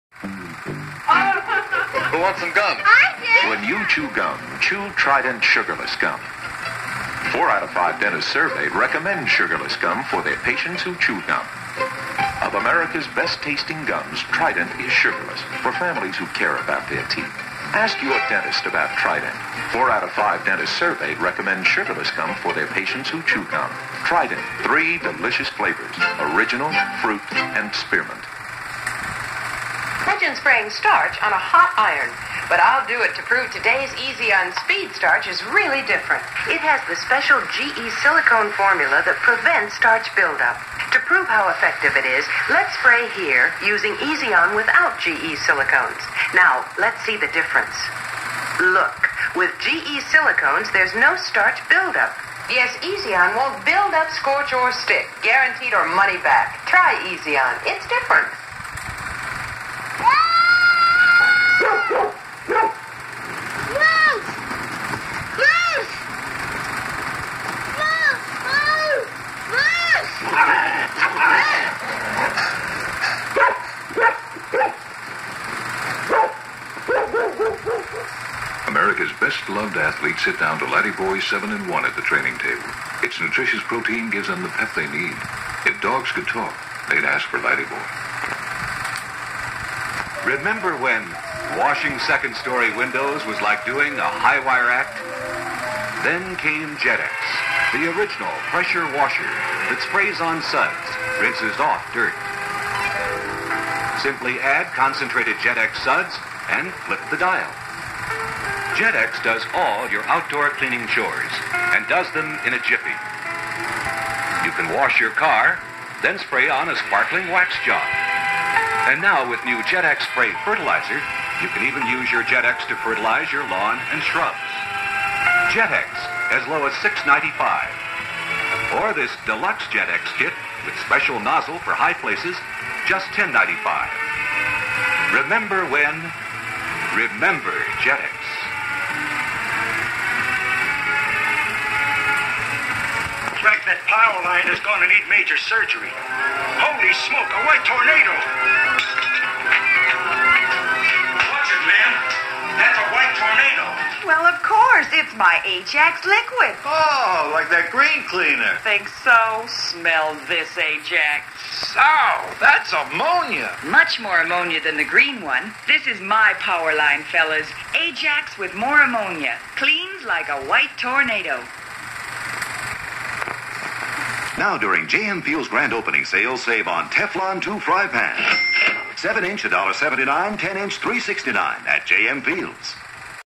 16mm Film: Classic Color Commercials (Pt. 15)
The commercials featured in this video are Trident gum, Easy On, Laddie Boy dog food, Jet-X, Ajax liquid (white tornado) and JM Fields department store.